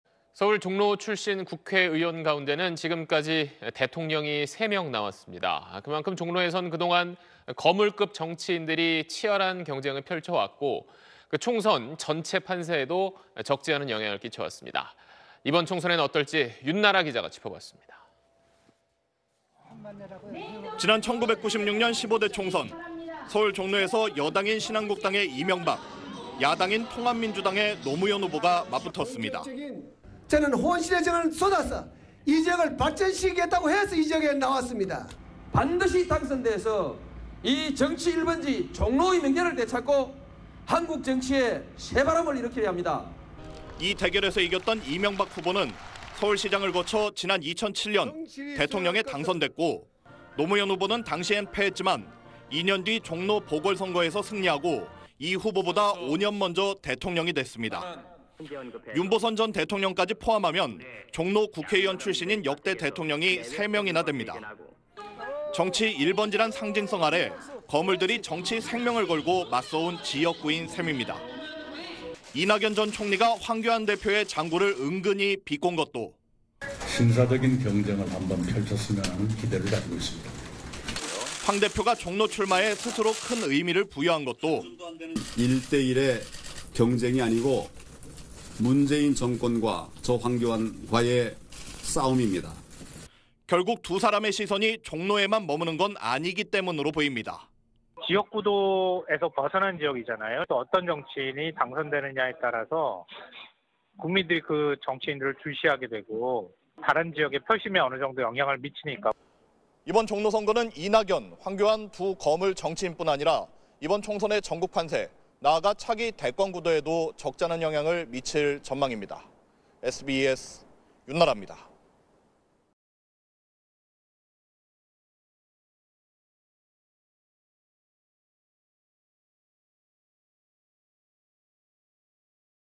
대통령 3명 배출한 '서울 종로'…미리 보는 대선? / SBS
서울 종로 출신 국회의원 가운데 지금까지 대통령이 3명 나왔습니다. 그만큼 종로에서는 그동안 거물급 정치인들이 치열한 경쟁을 펼쳐왔고, 총선 전체 판세에도 적지 않은 영향을 끼쳐 왔습니다.
이번 총선에는 어떨지 윤나라 기자가 짚어봤습니다.